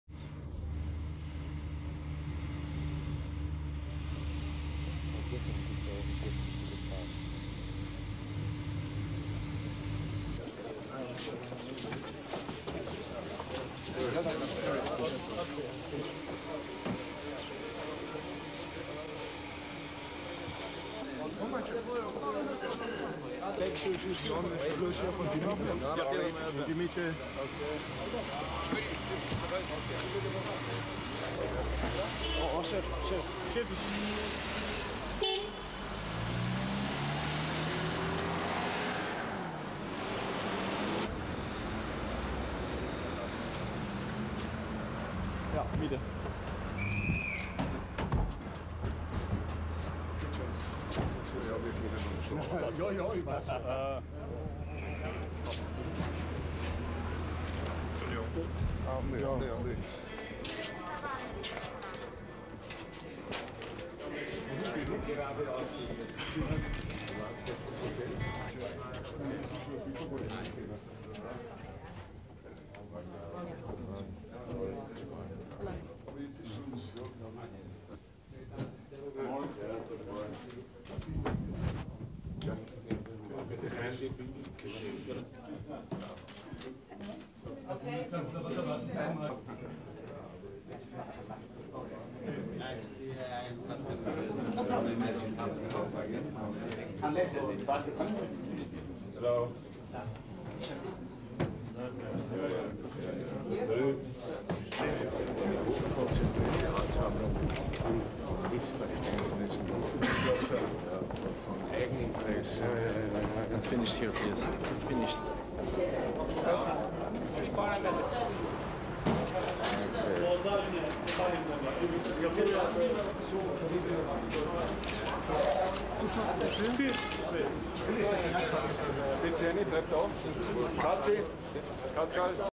Albania - Arrival OSCE envoy Franz Vranitzky
(8 Mar 1997) T/I 10:14:16

Former Austrian Chancellor Franz Vranitzky, heading a delegation from the Organisation for Security and Cooperation in Europe, met Albanian President Sali Berisha on Saturday (8/3) for talks on how to end an armed rebellion in southern Albania.

SHOWS:

TIRANA, ALBANIA, 8/3

Franz Vranitzky, OSCE envoy and former Austrian chancellor, walking away
from plane with entourage;
CU Vranitzky walking past into airport building;
exterior building;
Vranitzky out of building into car;
motorcade leaving airport;
Vranitzky out of car and into hotel;
Vranitzky walking in lobby,
shaking hands with officials;
VS round table with Vranitzky (Albanian President Sali Berisha not
visible);
Vranitzky and entourage walking in lobby;
Vranitzky leaving hotel and into car.

Runs 2.13